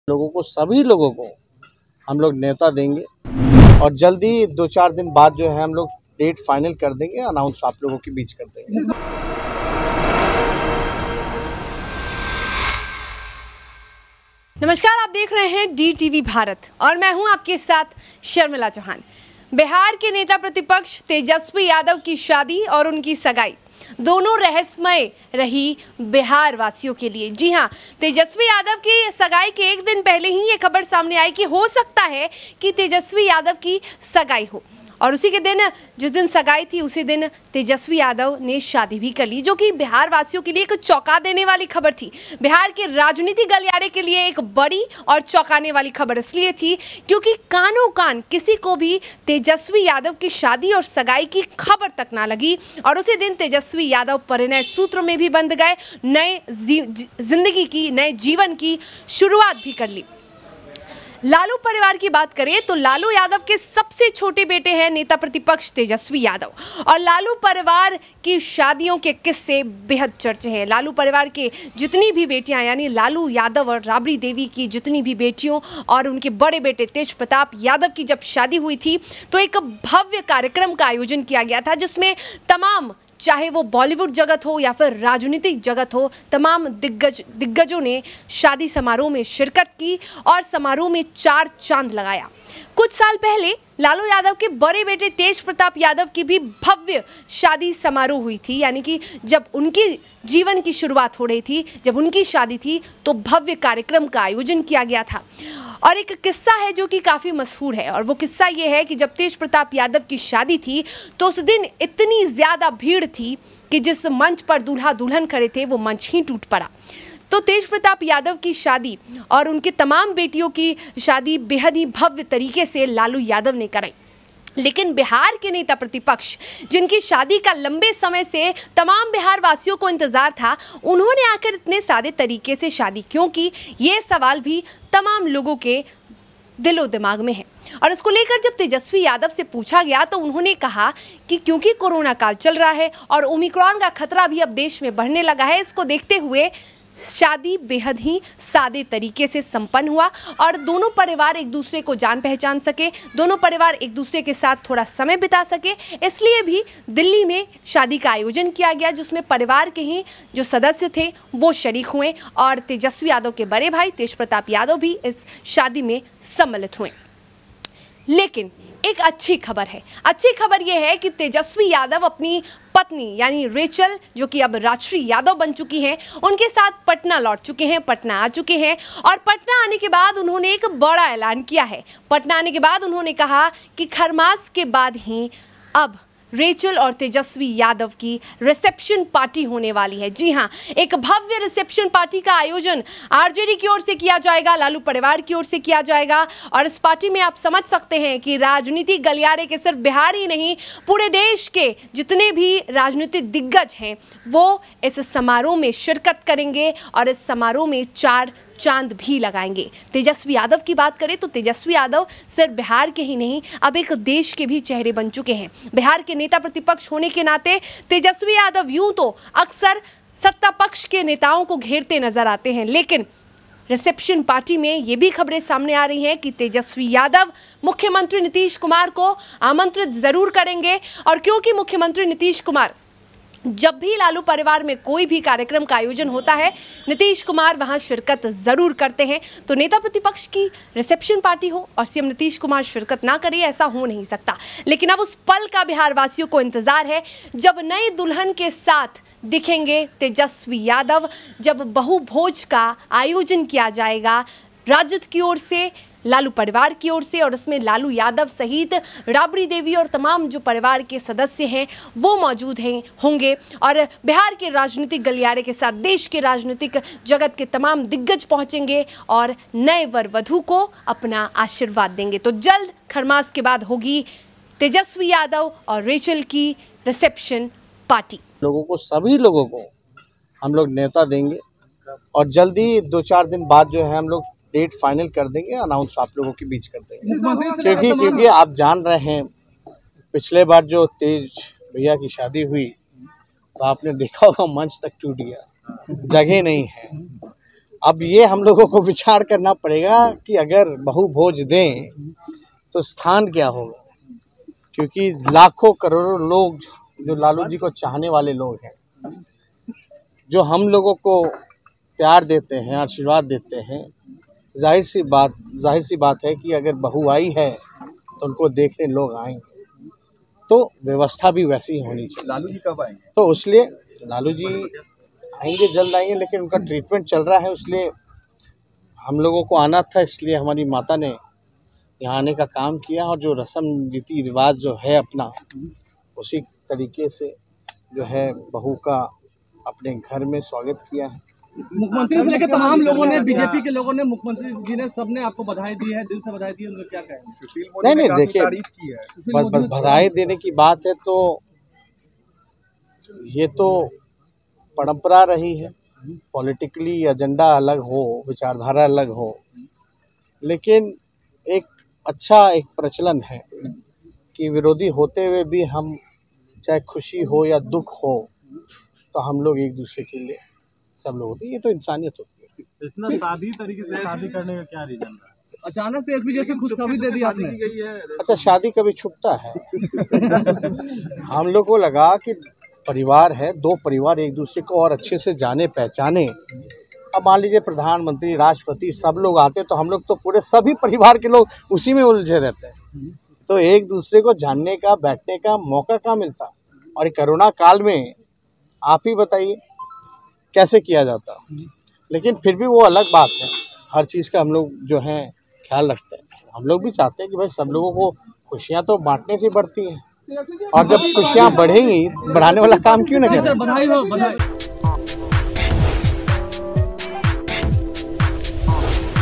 TEJASHWI ने CLEAR किया अपने RECEPTION की तारीख, जानें कब मिलेगा भोज ।
TEJASHWI ने CLEAR किया अपने RECEPTION की तारीक, जानें कब मिलेगा भोज ।